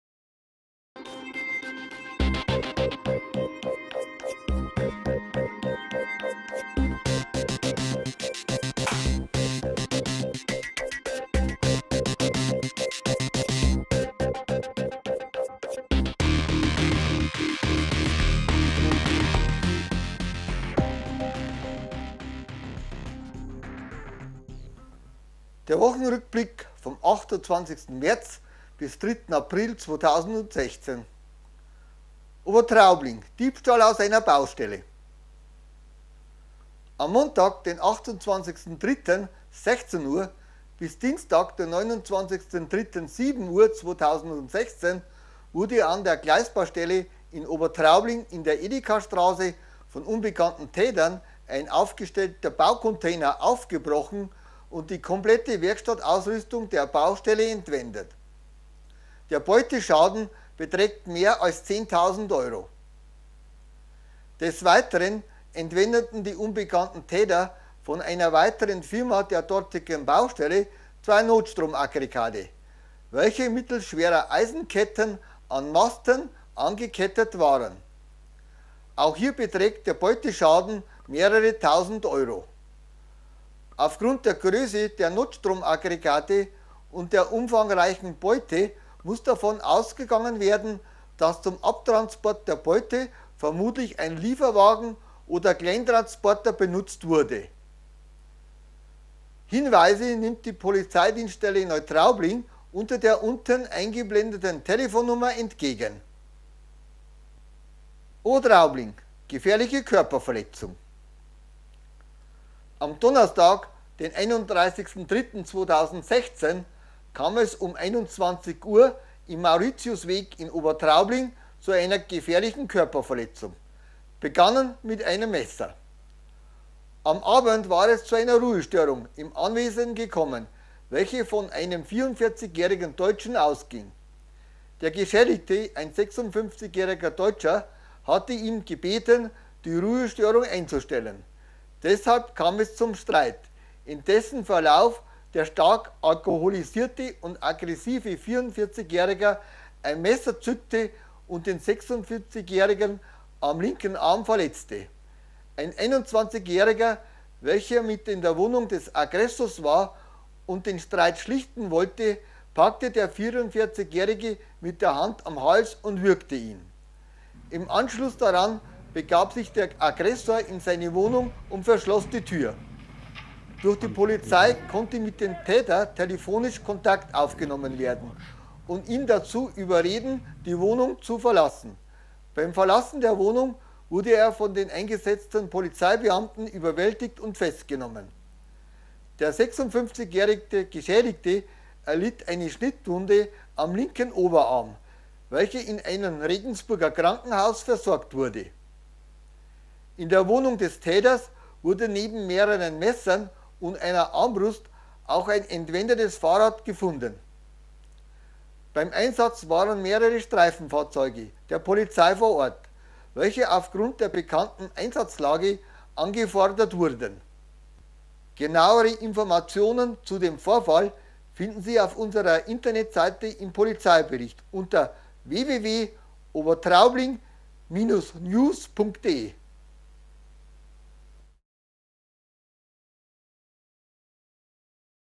Der Wochenbericht vom 29. 03. bis 03. 04. 2016
Der Wochenbericht aus Obertraubling vom 29. 03. bis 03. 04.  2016:
Großeinsatz der Polizei:  - Polizei überwältigt Mann nach Messerattacke in Obertraubling: